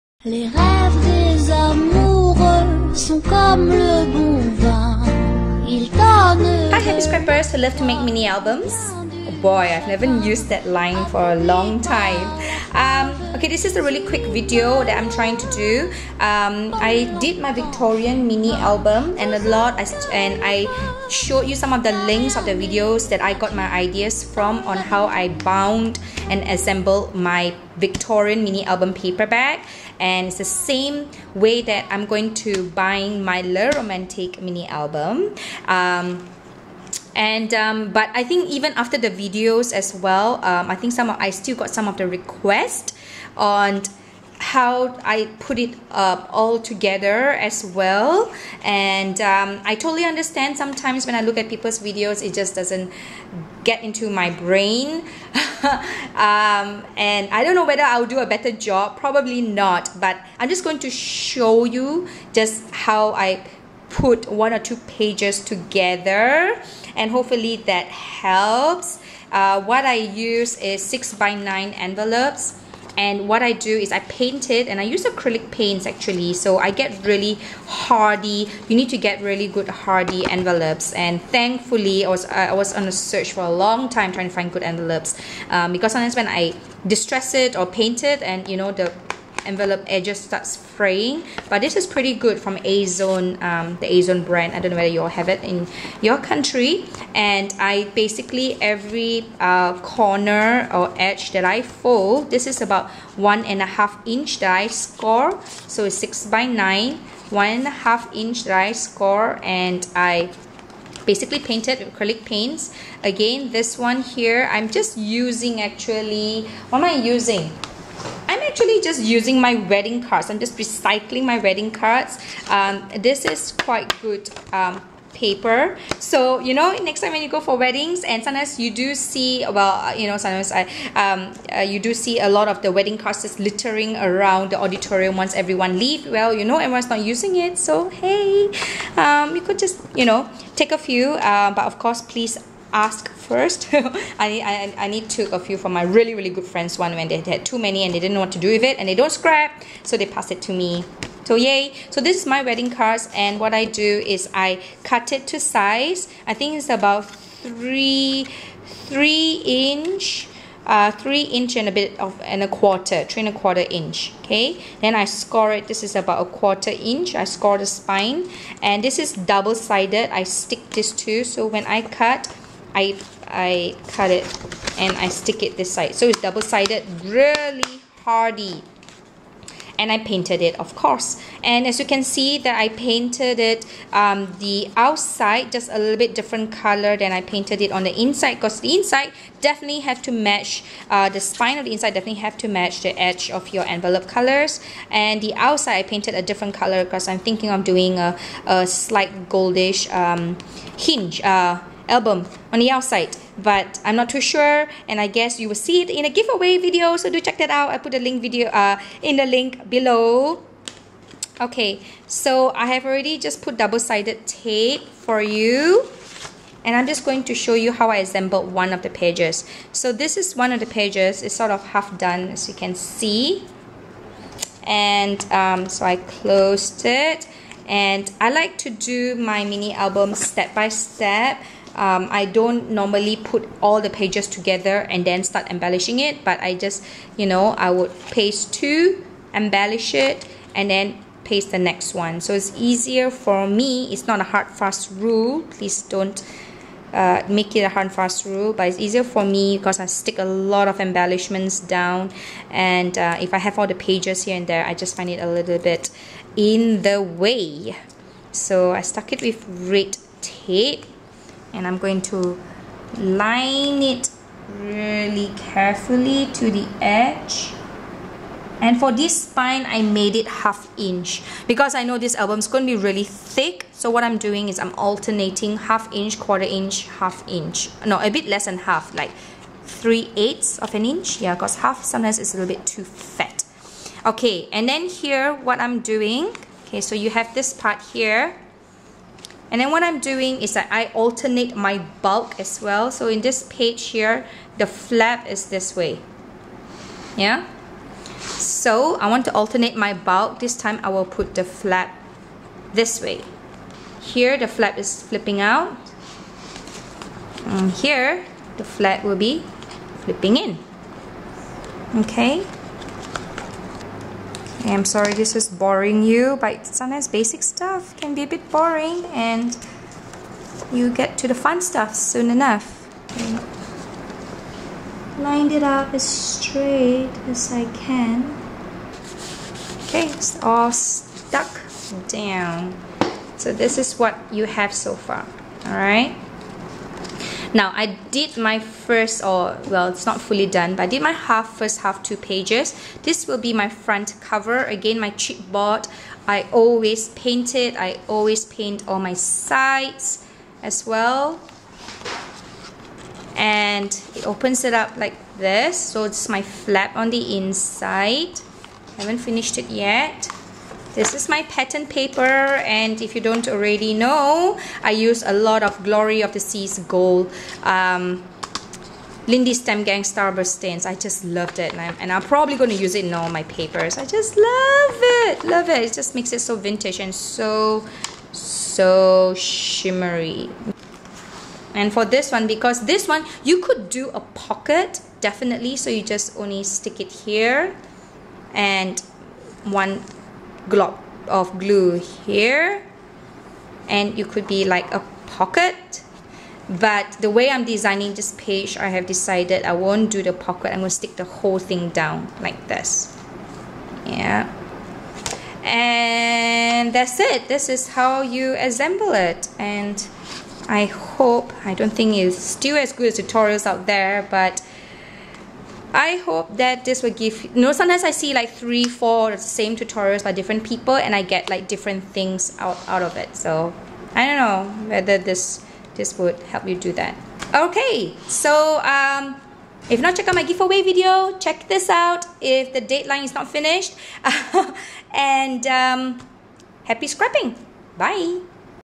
Le Romantique Tutorial Series - 3: How i bind my Mini Album, Envelopes Assembly
This is part 3 of my Tutorial series in my Le Romantique Wedding Mini Album.

In this tutorial i show you how I bind my mini album, this is the similar technique which i used to bind my Victorian Garden Cottage Mini Album

Though i did a tutorial on how i bind my Victorian Cottage Mini Album, i still had questions on how i adhere everything altogether. Guess my first tutorial was not that great huh??.

SO here is my second attempt to show you on how i assemble my paper bags. I get this idea on Paper Assembly some from Kathyhorta and some from Kathryn.

In the Le Romantique i mainly used  6x9inches envelopes. I forgot to give the measurements in this video but i did give detailed measurements in my first tutorial

I made this Wedding Mini Album from the Graphic 45 Le Romantique papers, DCWV La Creme, and DCWV Luxury Papers, i also used abit of the G45 Once Upon a Springtime. You can see the Mini album in this link:

I did a few tutorials on some of the pages and techniques that i used in this Wedding Mini Album-:

3) How i assembled the mini album (This is the Tutorial)
4) Tutorial Pocket Envelope (on the Gents Page) (Pending Upload)
5) How i made my celebration page and vintage suitcase (Pending Upload)
6) How i made my front cover and the spine of my mini (Pending Upload)